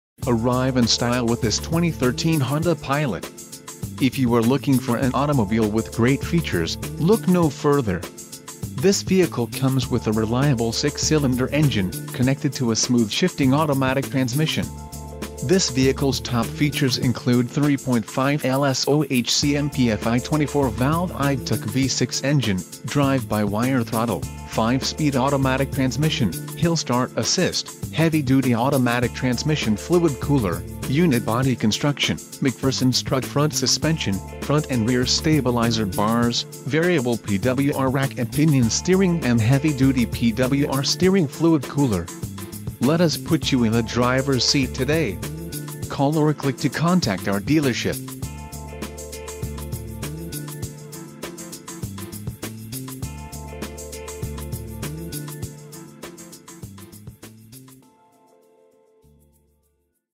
2013 Honda Pilot Touring 4x4 NAVI RES - Washington Honda ...
*** 1 OWNER *** CLEAN AUTO CHECK... 4X4 V6 MOON ROOF LEATHER ALLOYS ** NAVIGATION ** REAR ENTERTAINMENT ** BACK UP CAMERA!!!